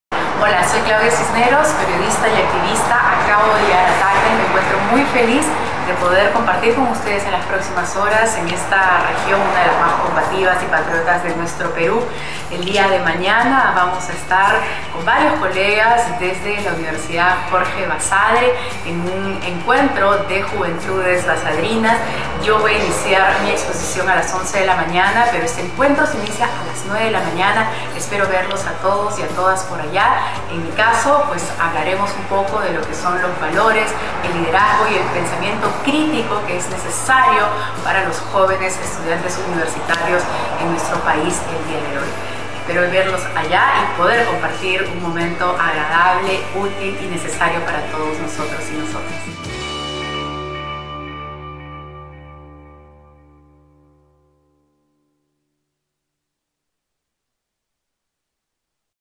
PERIODISTA CLAUDIA CISNEROS ESTARÁ PRESENTE EN "ENCUENTRO CON LA JUVENTUD BASADRINA"
"Encuentro con la Juventud Basadrina" se realiza como parte de nuestro 45º Aniversario Institucional.